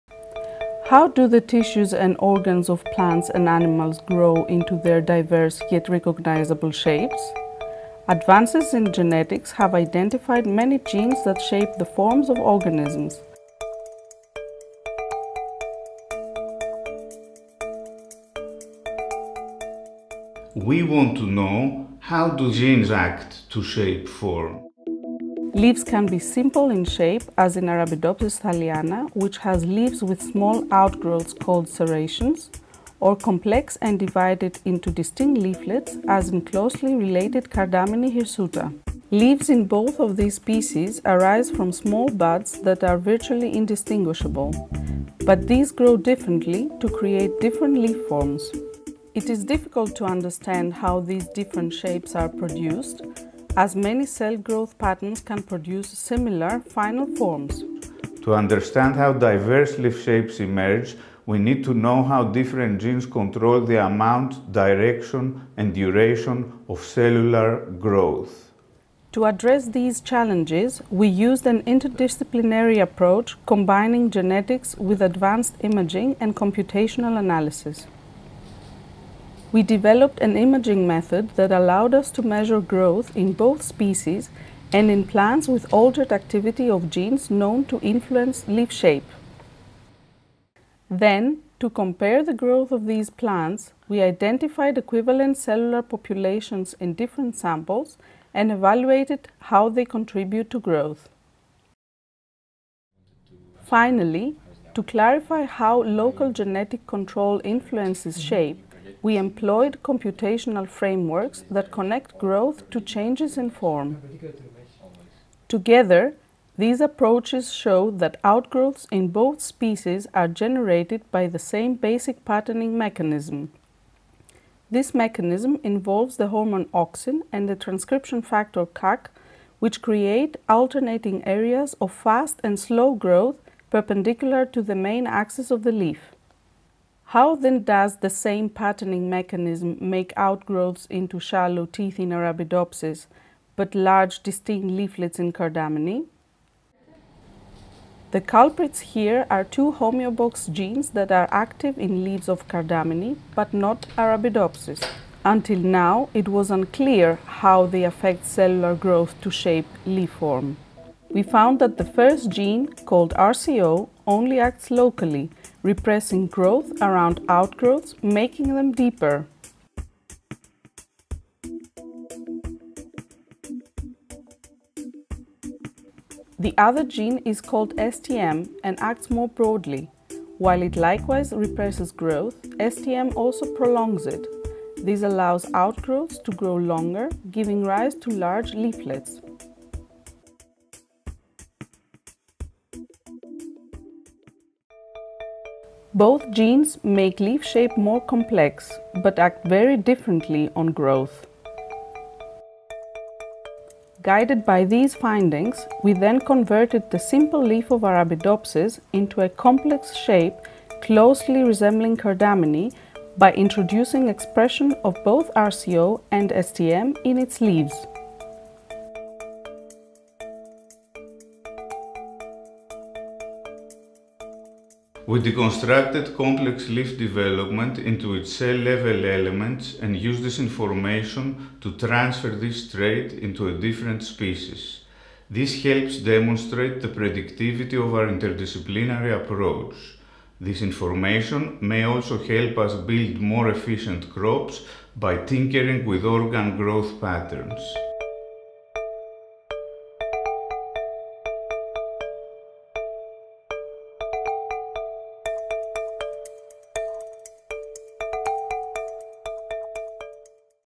Leaf Shape-Shifters / Cell, May 30, 2019 (Vol. 177, issue 6)
How do genes act in concert to shape form? Tsiantis and colleagues use a combination of approaches to deconstruct the impact of growth and patterning on leaf morphology and convert the leaf shape of A. thaliana to that of another plant species with a more complex morphology.

D. Kierzkowski, A. Runions, F. Vuolo, S. Strauss, R. Lymbouridou, A.-L. Routier-Kierzkowska, D. Wilson-Sanchez, H. Jenke, C. Galinha, G. Mosca, Z. Zhang, C. Canales, R. Dello Ioio, P. Huijser, R.S. Smith, and M. Tsiantis. (2019). A growth-based framework for leaf shape development and diversity. Cell 177.